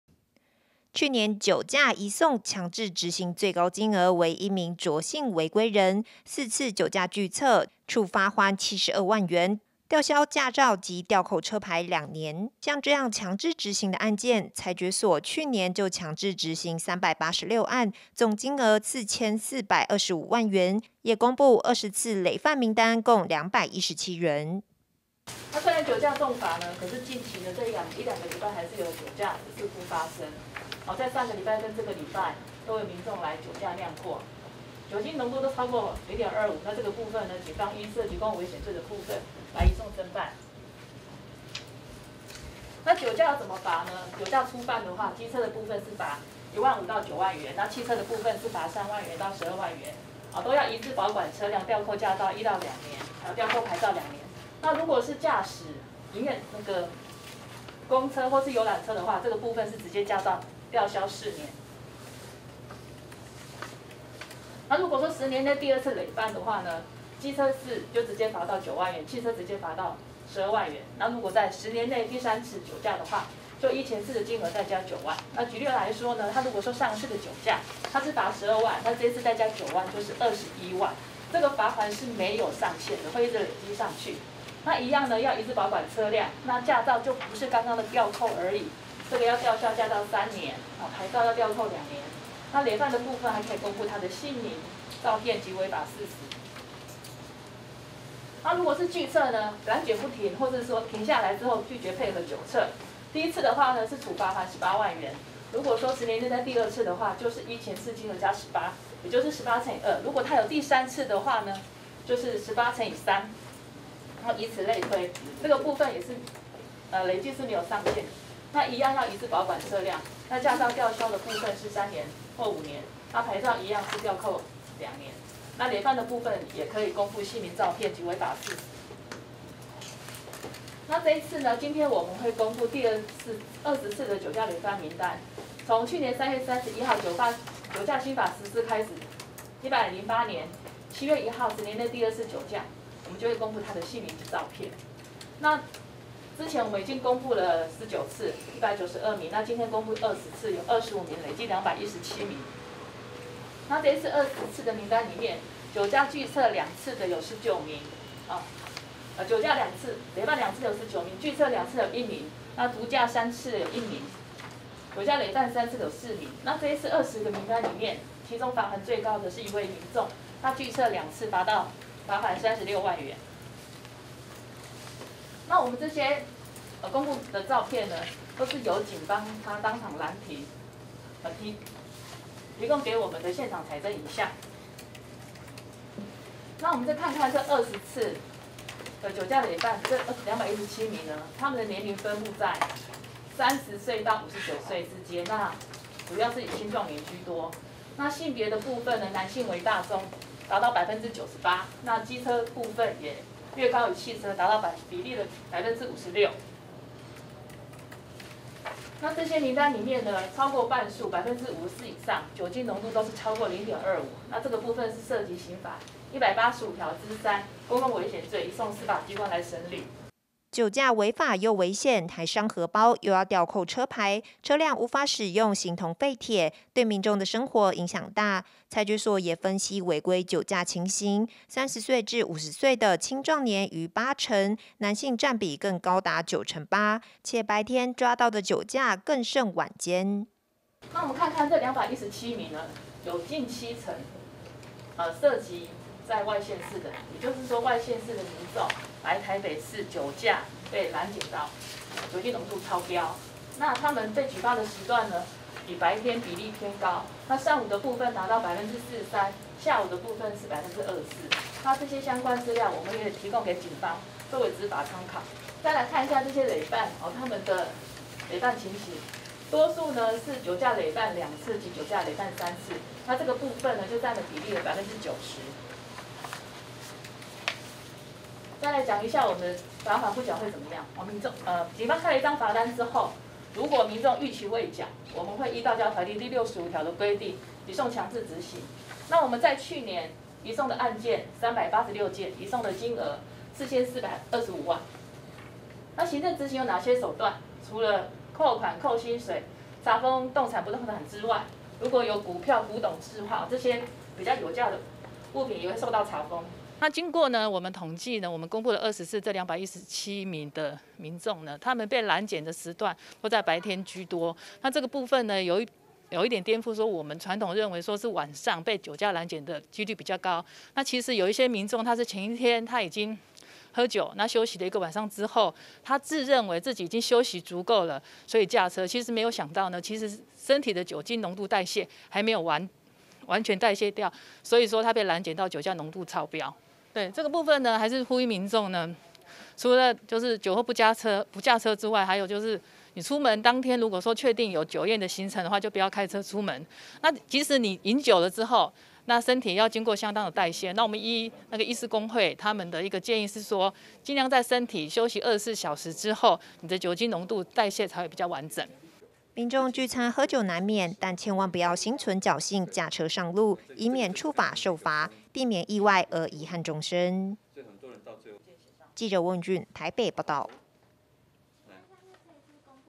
酒駕|肇事駕駛|車牌|打擊酒駕！北市公布25名酒駕累犯照片
打擊酒駕！北市公布25名酒駕累犯照片

酒駕新規去年3月31日上路，台北市交通事件裁決所迄今公布20次累犯名單共217人，且貫徹酒駕零容忍政策，罰鍰逾期未繳移送強制執行，累犯還會公布姓名照片，違規車輛一律吊扣牌照2年，盼民眾切勿以身試法。

去年酒駕移送強制執行最高金額為一名卓姓違規人，4次酒駕拒測，處罰鍰72萬元、吊銷駕照5及吊扣車牌2年，像這樣強制執行的案件，裁決所去年就強制執行386案，總金額4425萬元，也公布20次累犯名單共217人。

酒駕違法又危險，還傷荷包，又要吊扣車牌，車輛無法使用形同廢鐵，對民眾的生活影響大。裁決所也分析違規酒駕情形， 30歲至50歲的青壯年逾8成，男性占比更達9成8，且白天攔停比例更甚晚間。

民眾聚餐喝酒難免，但千萬不要心存僥倖駕車上路，以免觸法受罰，避免意外而遺憾終身。